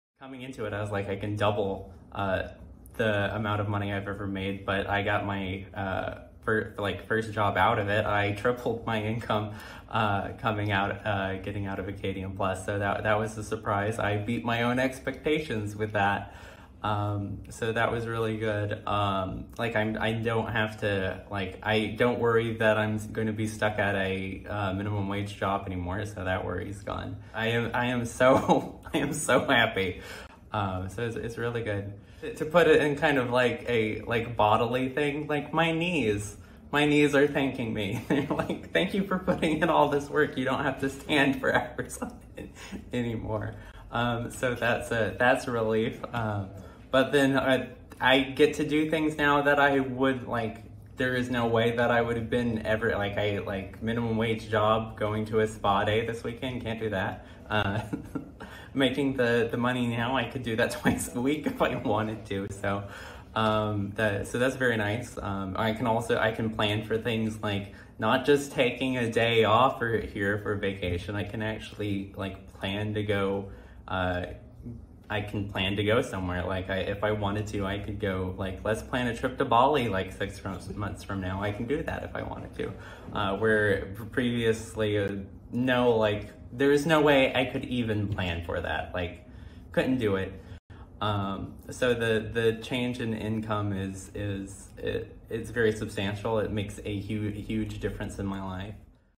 Teaser: How Michael Hodges 3X his income
Michael Hodges shares how he 3X his income by making the career switch from events to digital marketing. Watch this space to watch his full story